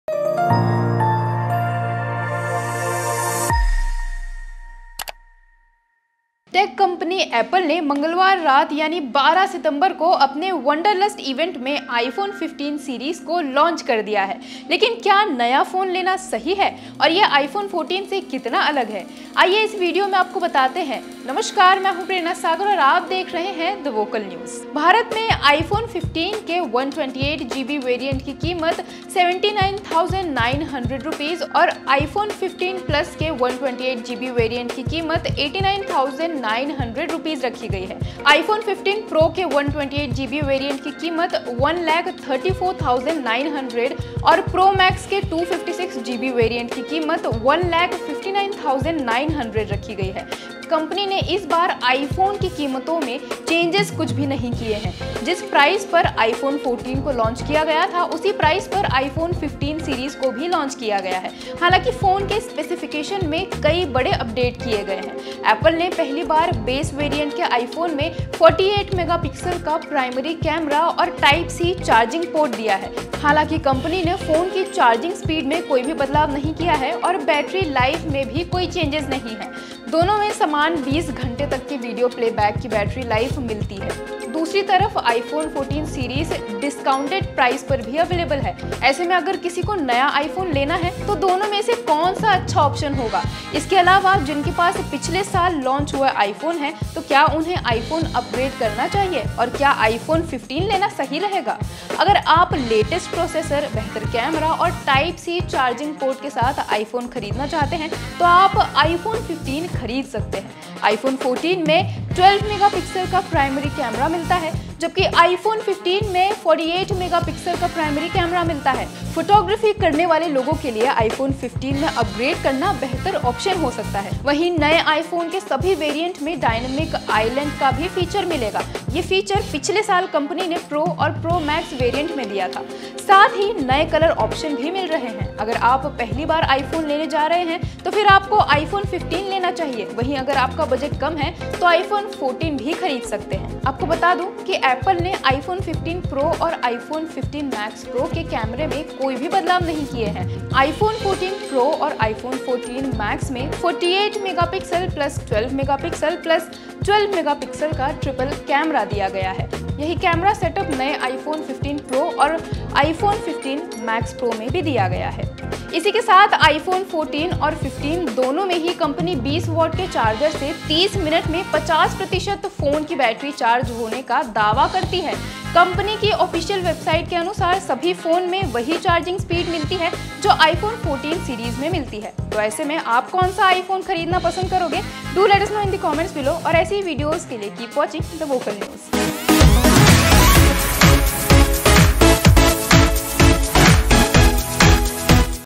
Apple Wonderlust Event: Iphone 15 Iphone 14 से कैसे है अलग?
Anchor by Prerna Sagar
Edit by Vansh